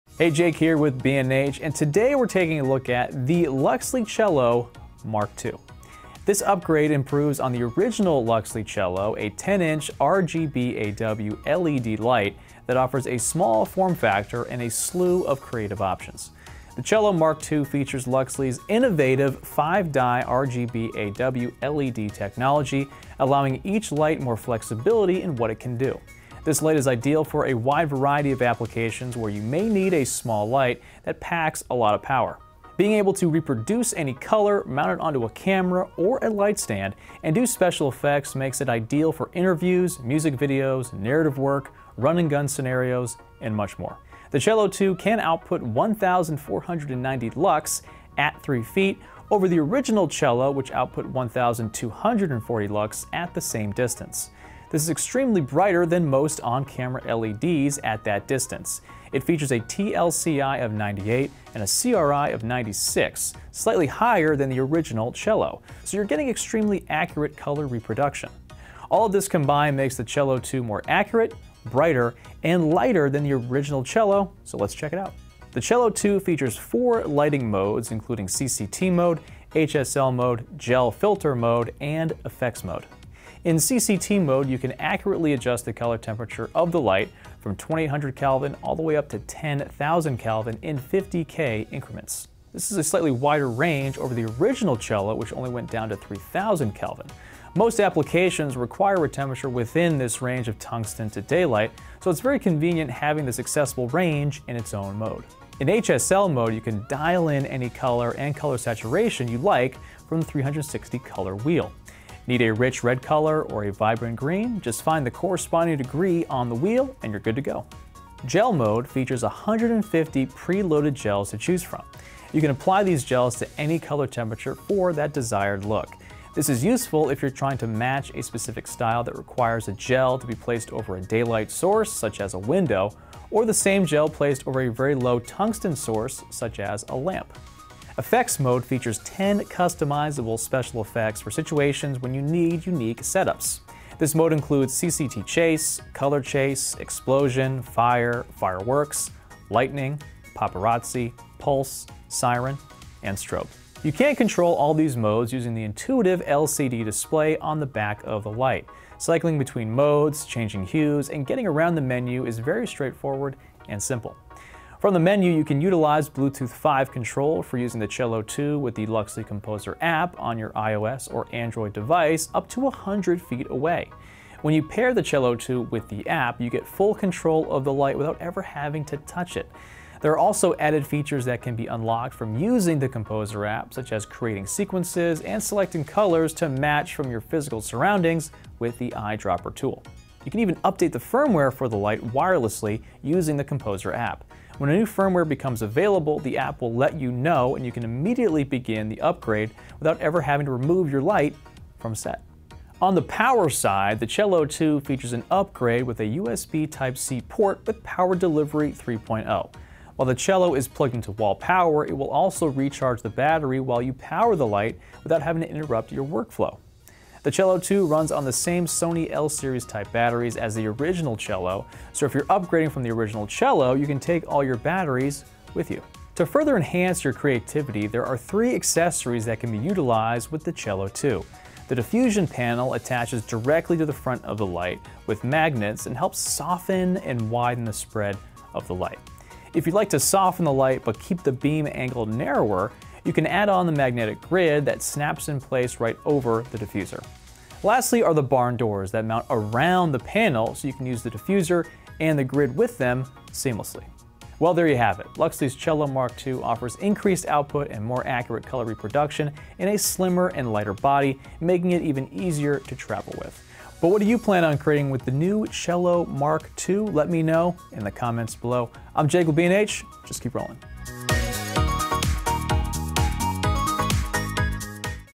Luxli Cello 2 | First Look
We’re taking a look at the Luxli Cello Mark 2. This on-camera RGBAW LED light panel is small but packs a lot of power, with the ability to output 1,490 lux at 3 feet, a TLCI of 98, a CRI of 96, a color temperature range of 2800K to 10,000K and much more! Would you add this to your video lighting setup?

More Luxli products
- Luxli Fiddle Pocket RGBAW LED Light
- Luxli Taiko 2x1 LED Light
- 10 Creative Ways to Use a Color-Changing LED Light | Luxli Timpani